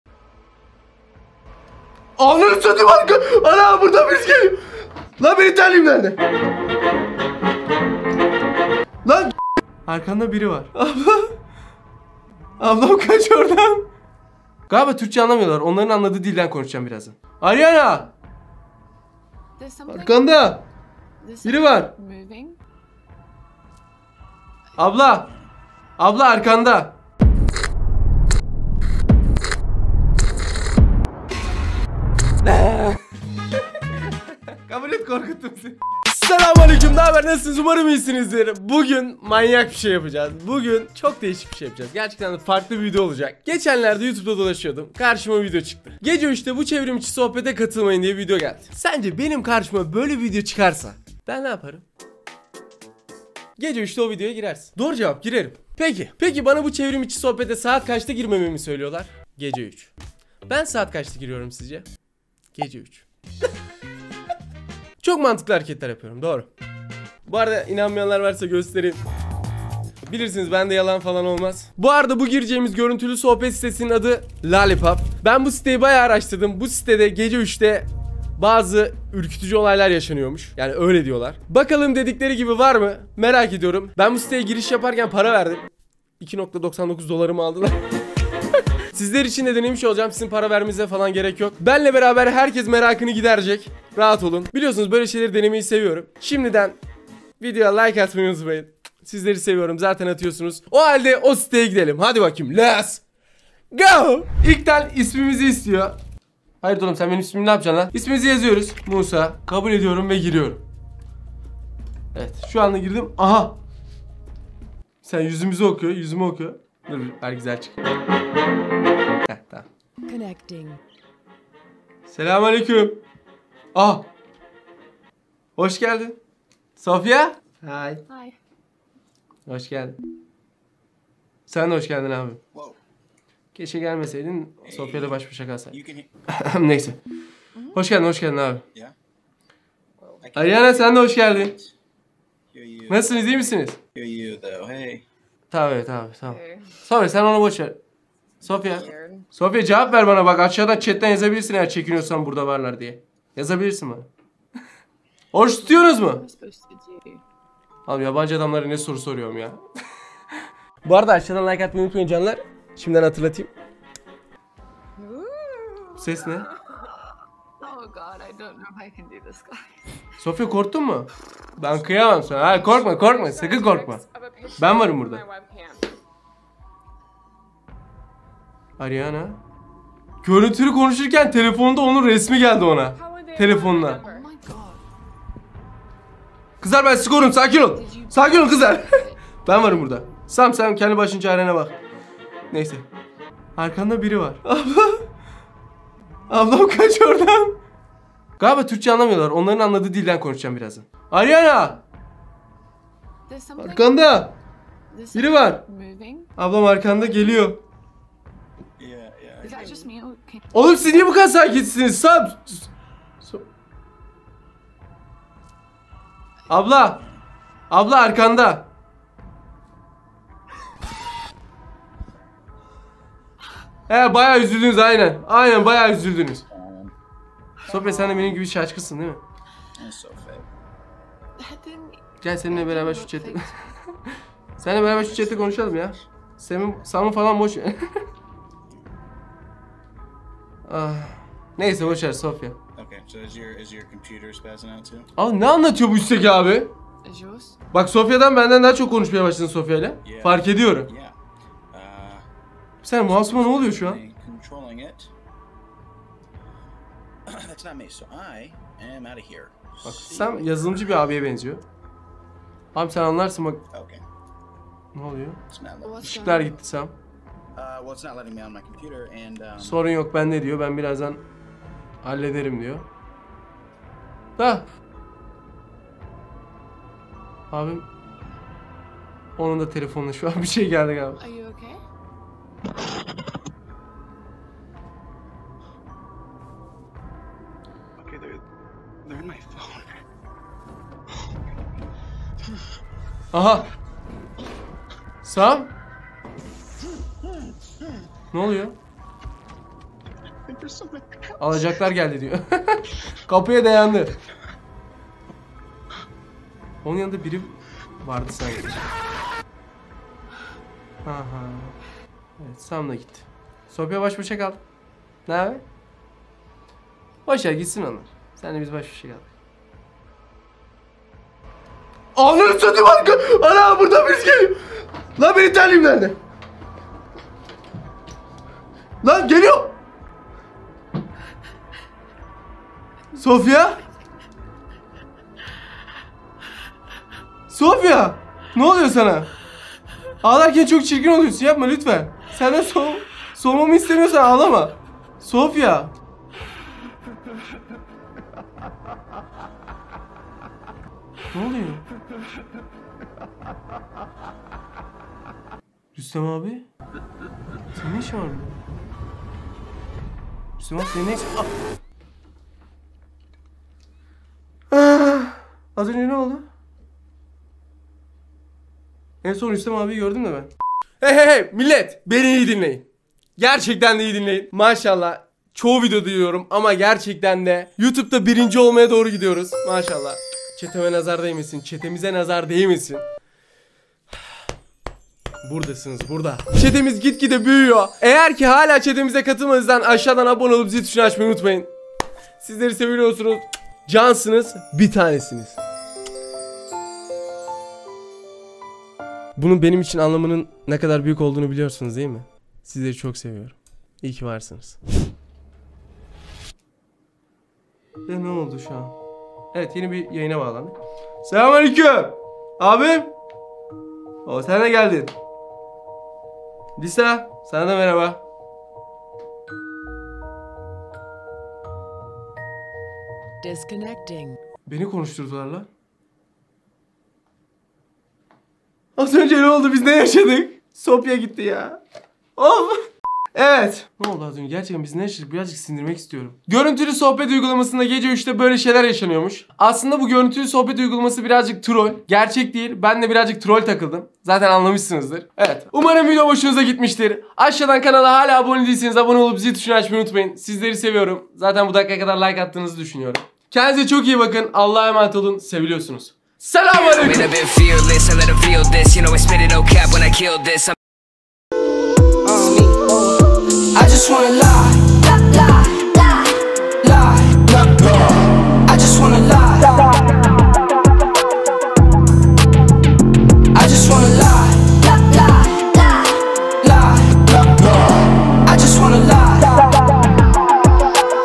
GECE 3'te Bu GÖRÜNTÜLÜ KONUŞMAYA KATILMAYIN ! *Bilgisayarım Hacklendi*
Ben Musa,Bu videoda GECE 3'te Bu GÖRÜNTÜLÜ KONUŞMAYA KATILMAYIN ! *Bilgisayarım Hacklendi* Çok eğlenceli bir o kadar komik ve troll dolu bir oyun videosu oldu !! Sakın bu uygulamaya katılmayın. Daha fazla bu tarz aksiyon , ürkütücü ve korku dolu videolar gelsin istiyorsanız yorumlarda belirtmeyi unutmayın!! bu uygulamayı test ettim sizler için umarım video hoşunuza gitmiştir